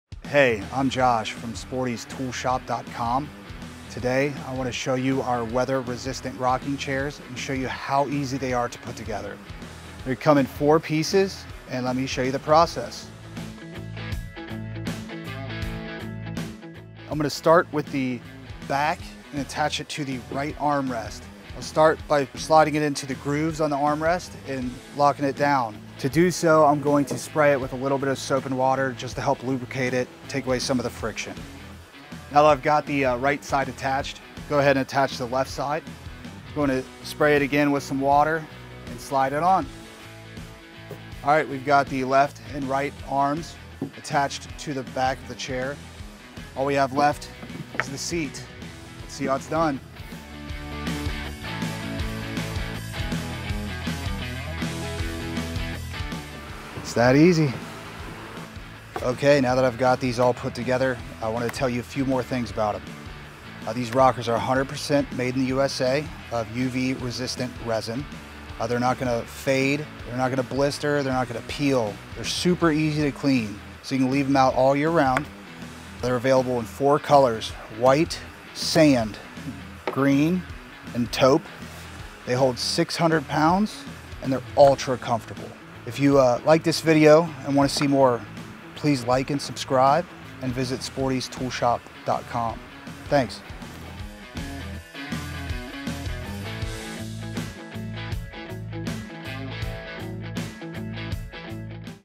How to Assemble The Weather Resistant Rockaway Rocker
Follow along as Josh, from Sporty'sToolShop.com, shows you some tips and tricks to easily assemble the Rockaway Rocker. Comfortable and maintenance free, these Rockers go together in a snap and provide years of comfortable use.